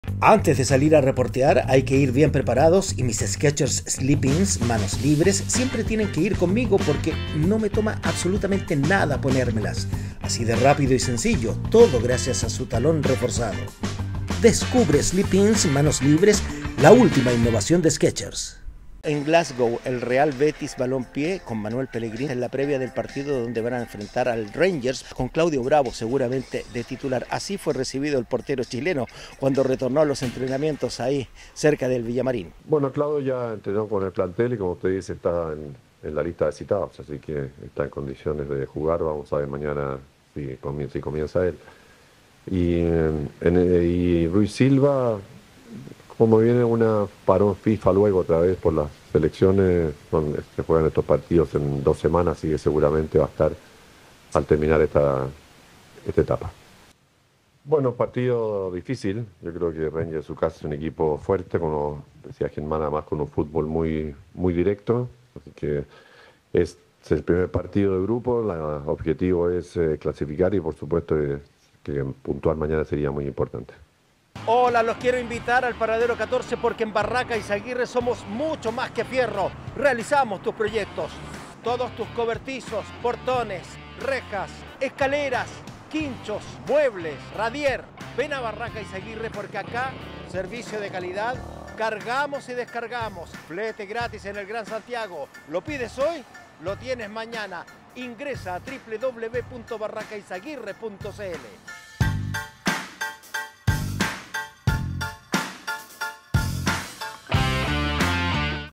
Previa de la EUROPA LEAGUE: PELLEGRINI adelanta que BRAVO está listo para volver a la TITULARIDAD
Con la presencia de dos chilenos el Betis debuta esta temporada en la Europa League como visitante ante Rangers.

El equipo de Claudio Bravo y comandado por Manuel Pellegrini viene de una dura goleada el fin de semana, por lo que en el certamen europeo busca recuperar la memoria ante uno de los grandes de Escocia.

Toda la previa del partido con Eugenio Salinas, Kenotrotamundos en RedGol, la palabra del Ingeniero en Glasgow, donde se refirió a la situación de Bravo y a la de su colega en el arco Rui Silva quien tiene fecha de retorno.